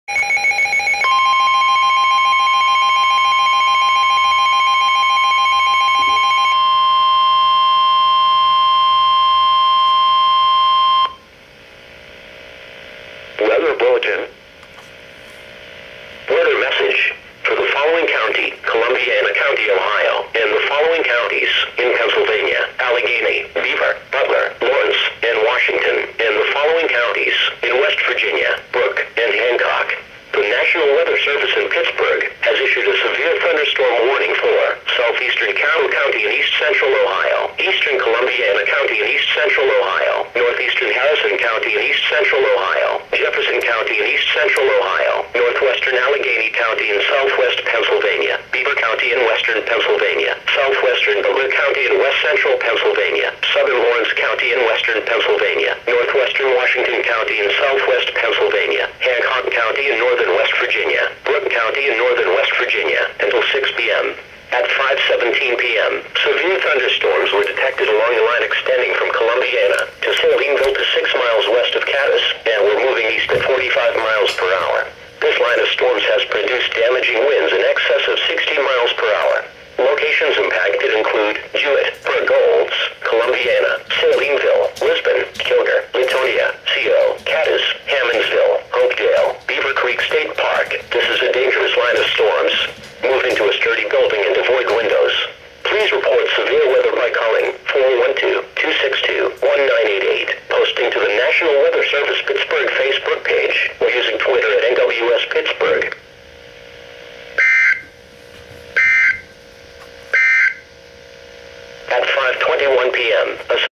Severe T-Storm Warning [EAS #243] 7/10/13
Station: KIH-35 Pittsburgh, PA
Issued by: NWS Pittsburgh, PA